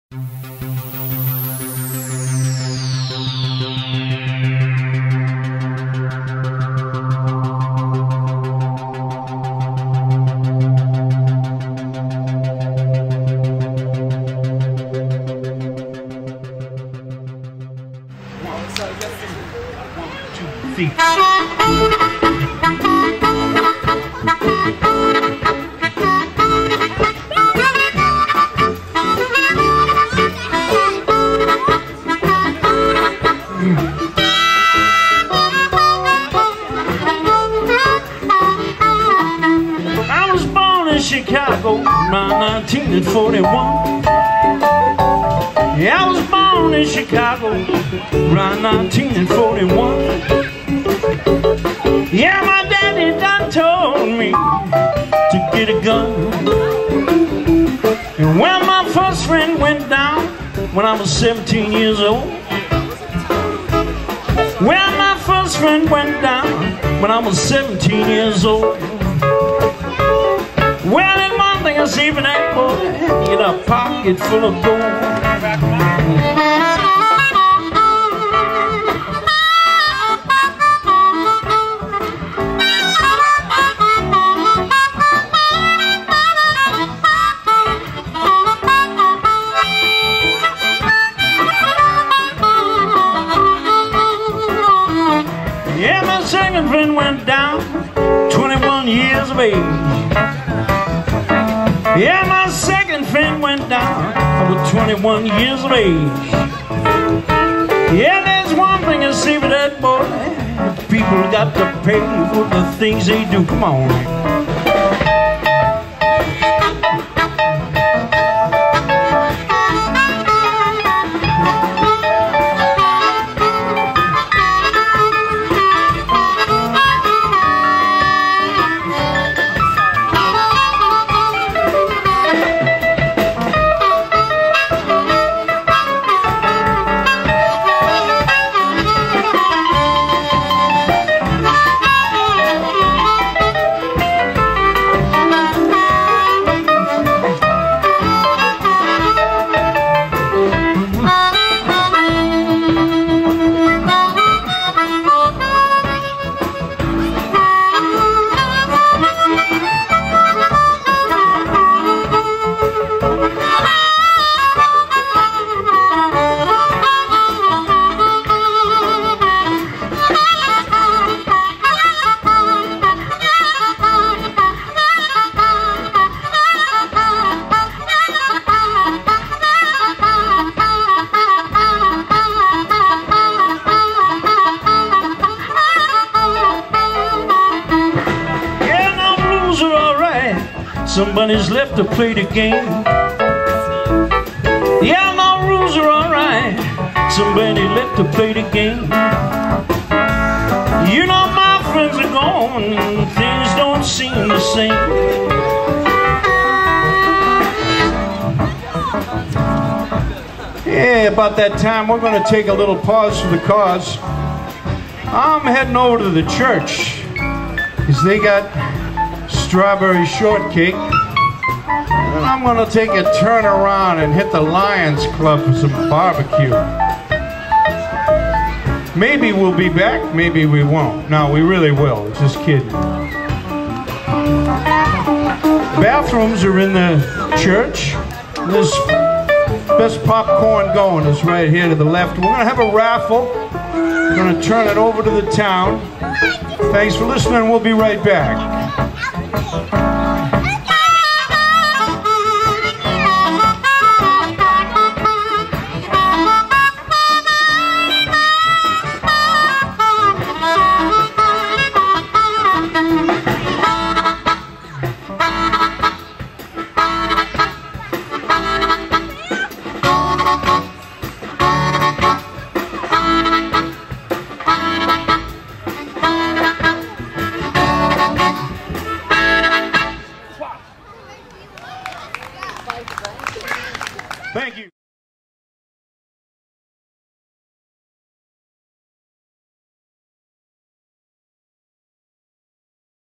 Hatrack Gallagher & the Ric Maure Trio (Walpole Common)
Here's a song by Hatrack Gallagher & the Ric Maure Trio live at the Walpole Concert on the Common series from July 12th 2011.